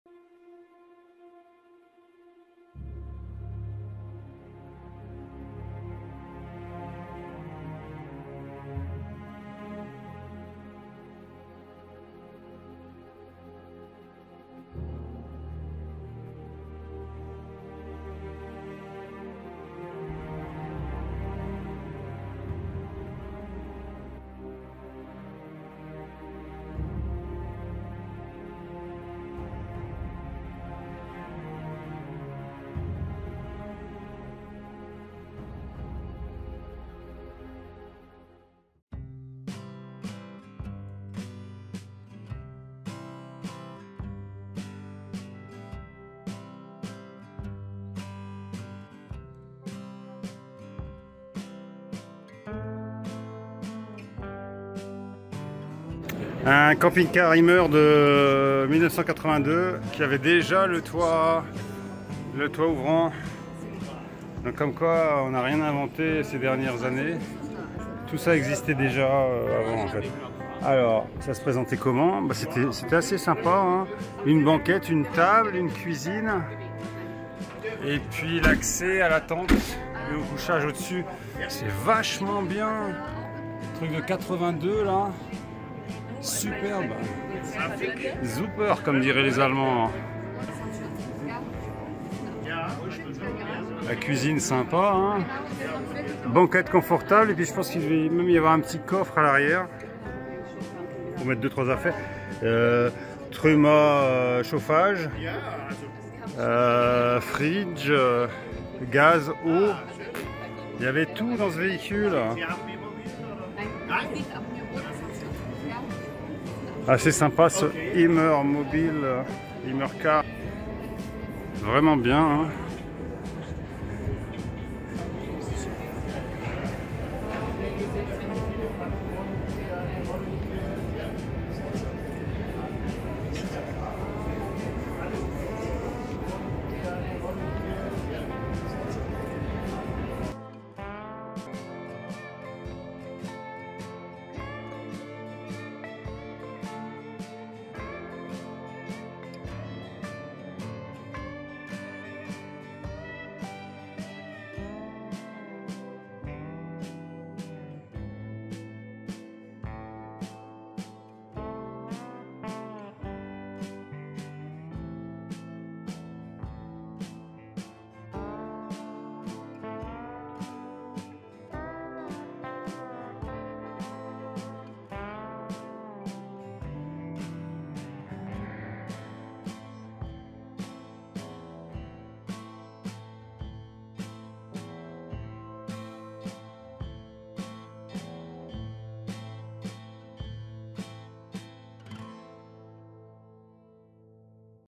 The ErwinHymer Museum - Hymercar 1982 - CMT Stuttgart, 2023
The Erwin Hymer Museum presents, as part of the CMT Stuttgart 2023, its famous van Hymercar, new for 1982!!! And announces two Fairs-Events in May and August 2023...
Did you miss the 1982 one? Nothing is lost, run...

Das Erwin-Hymer-Museum präsentiert im Rahmen der CMT Stuttgart 2023 seinen berühmten Van Hymercar, neu aus dem Jahr 1982!!! Und kündigt zwei Messen-Events im Mai und August 2023 an...
Haben Sie das von 1982 verpasst? Nichts ist verloren, lauf...

L'Erwin Hymer Museum presenta, nell'ambito del CMT Stuttgart 2023, il suo famoso furgone Hymercar, novità 1982 !!! E annuncia due Fiere-Eventi a maggio e agosto 2023...
Vi siete persi quello del 1982? Niente è perduto, corri...

El Museo Erwin Hymer presenta, en el marco de la CMT Stuttgart 2023, su famosa furgoneta Hymercar, novedad 1982!!! Y anuncia dos Ferias-Eventos en mayo y agosto de 2023...
¿Te perdiste el de 1982? Nada está perdido, corre..

Le Musée Erwin Hymer présente, dans le cadre du CMT Stuttgart 2023, son célèbre van Hymercar, nouveauté 1982 !!! Et annonce deux Foires-Evènements en Mai et Août 2023 …
Vous avez manqué celle de 1982 ? Rien n’est perdu, courez …